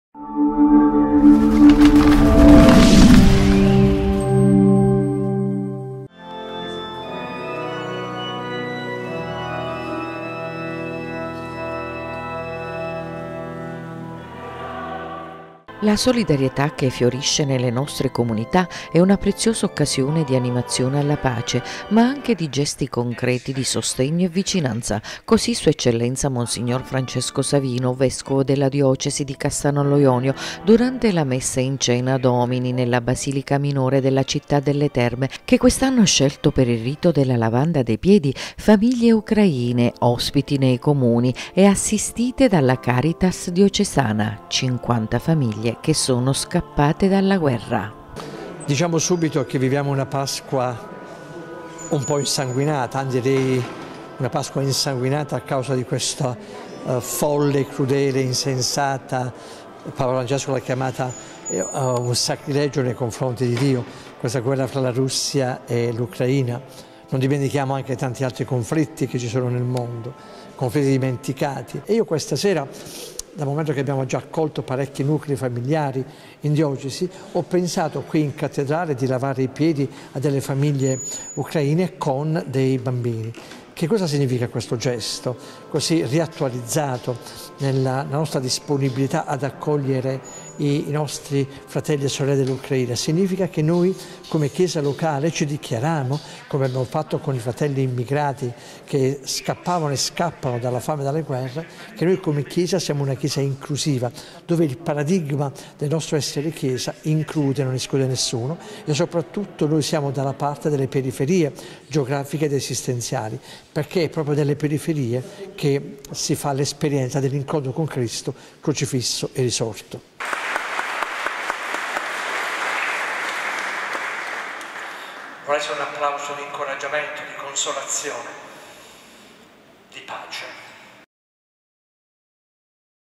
Cassano Jonio Il rito della lavanda dei piedi a famiglie ucraine
Cassano Jonio Il rito della lavanda dei piedi da parte di SE Mons Francesco Savino a famiglie ucraine scappate dalla guerra
15 aprile 2022.